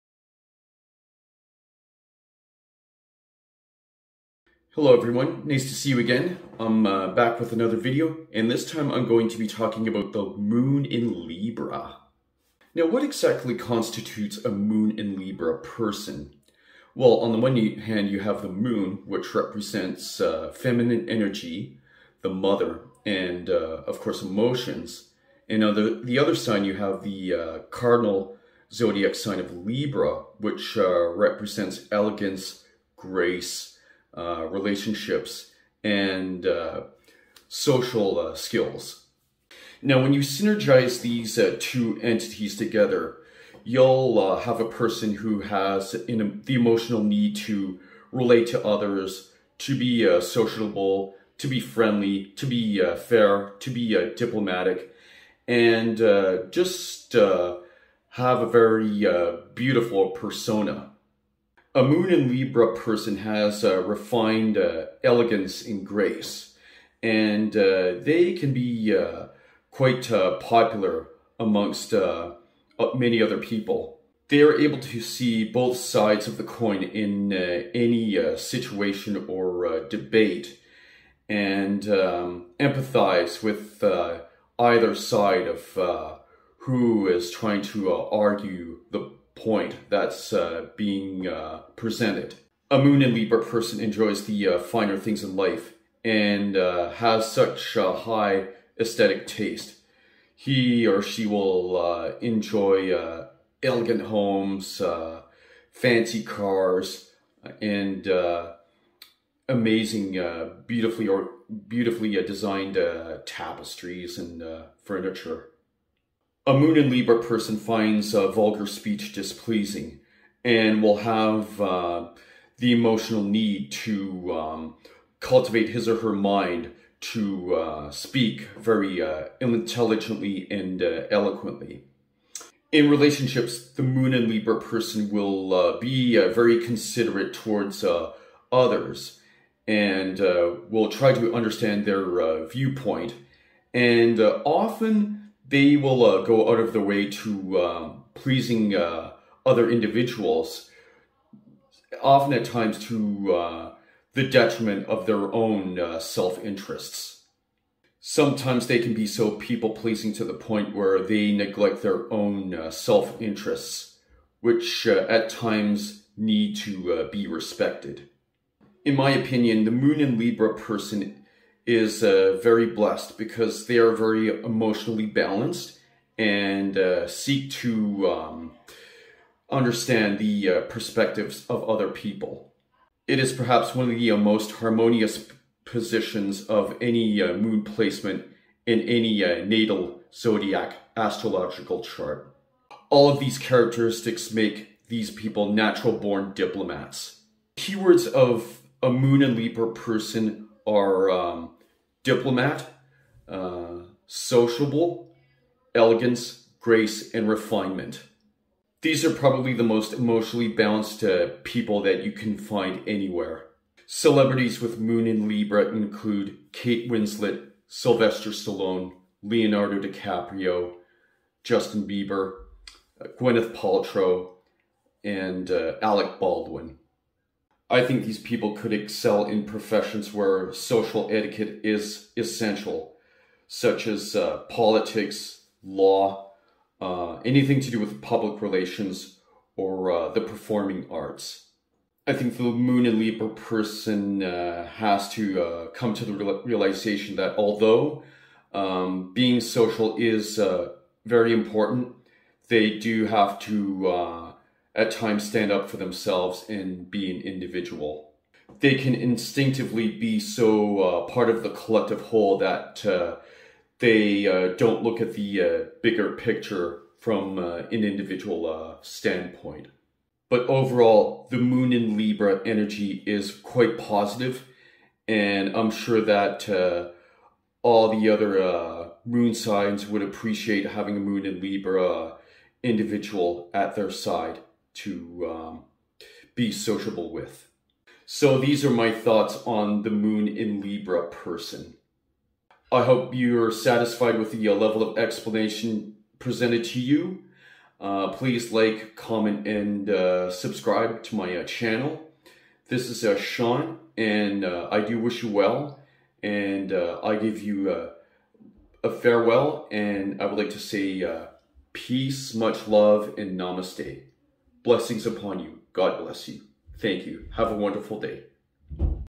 Moon in Libra Characteristics - From My Perspective
This is my explanation of what constitutes one with a natal Moon in Libra placement. Fundamental traits are diplomacy, elegance, grace and balance.